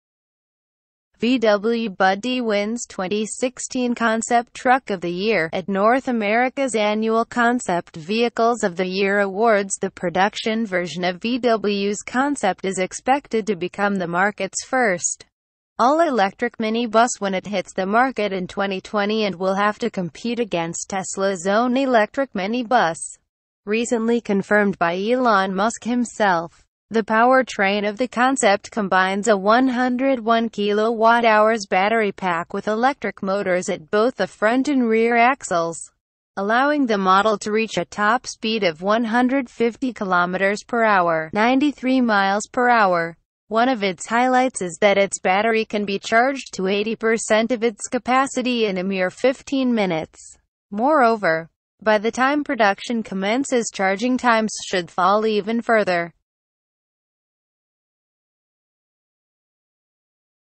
VW Budd-e Wins Truck of the Year' at North America's annual Concept Vehicles of the Year Awards
VW Budd-e Wins 2016 Concept Truck of the Year' at North America's annual Concept Vehicles of the Year Awards

The production version of VW's concept is expected to become the market's first all-electric minibus when it hits the market in 2020 and will have to compete against Tesla's own electric minibus, recently confirmed by Elon Musk himself.

The powertrain of the concept combines a 101 kWh battery pack with electric motors at both the front and rear axles, allowing the model to reach a top speed of 150 km/h (93 mph). More